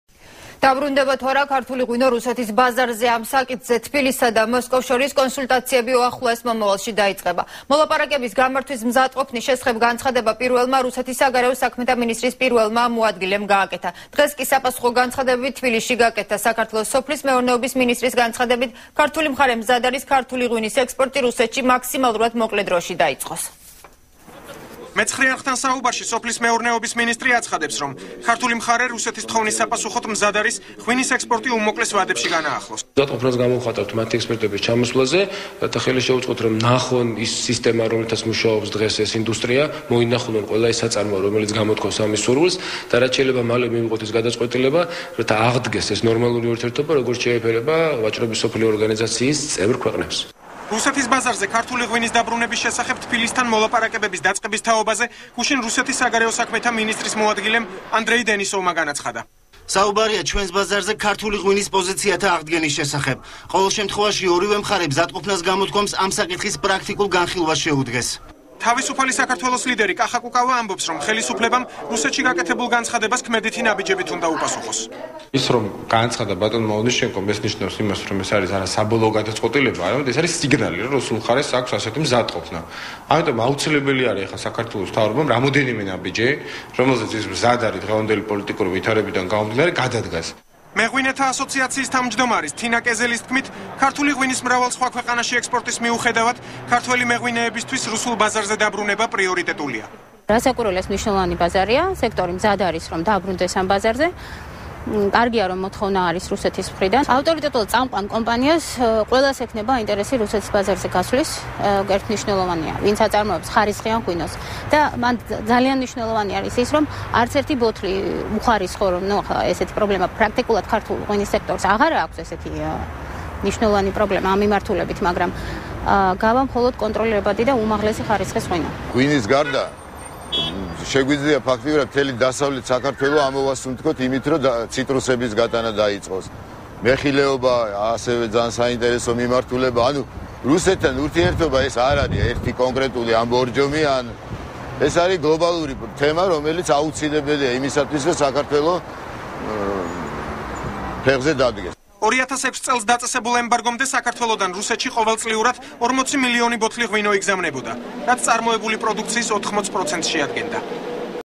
ახალი 9 | ქართული ღვინო რუსეთში | 15.11.12
დაბრუნდება თუ არა ქართული ღვინო რუსეთის ბაზარზე - ამ საკითხზე თბილისსა და მოსკოვს შორის მოლაპარაკებები უახლოეს მომავალში დაიწყება.  მოლაპარაკებების გამართვის მზადყოფნის შესახებ განცხადება პირველმა, რუსეთის საგარეო საქმეთა მინისტრის პირველმა მოადგილე ანდრეი დენისოვმა გააკეთა. დღეს კი საპასუხო განცხადებები თბილისში გაკეთდა. საქართველოს სოფლის მეურნეობის მინისტრის განცხადებით, ქართული მხარე მზად არის  ქართული ღვინის ექსპორტი რუსეთში მაქსიმალურად მოკლე დროში დაიწყოს.